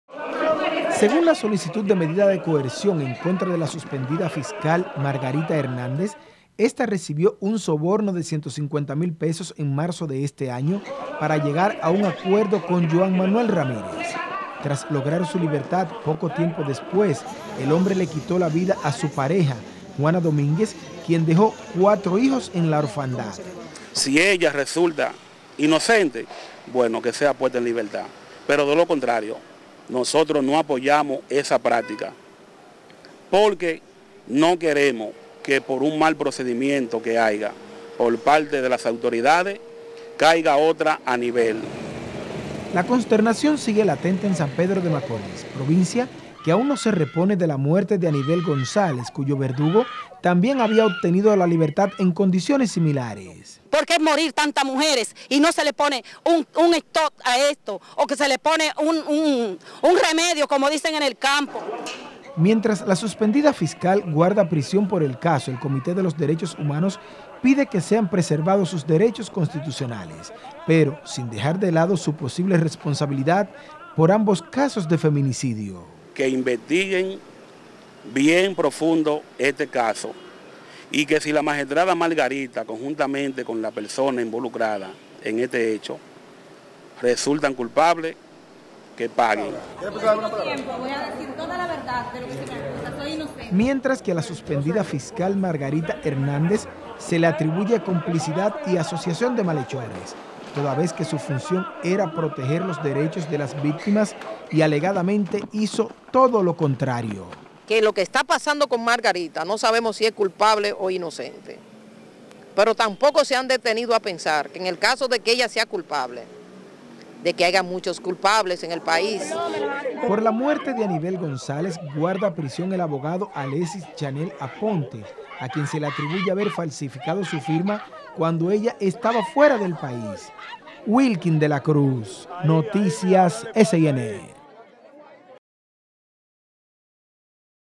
Feminicida habría pagado 150 mil a fiscal SPM por libertad, según solicitud medidas de coerción
El imputado Johan Manuel Ramírez, pagó 150 mil pesos a la fiscal  de la Unidad de Violencia de Género de San Pedro de Macorís suspendida, Margarita Hernández Morales, para que procediera a beneficiarlo con el acuerdo penal abreviado que permitió su excarcelación; según el documento de solicitud de medidas de coerción.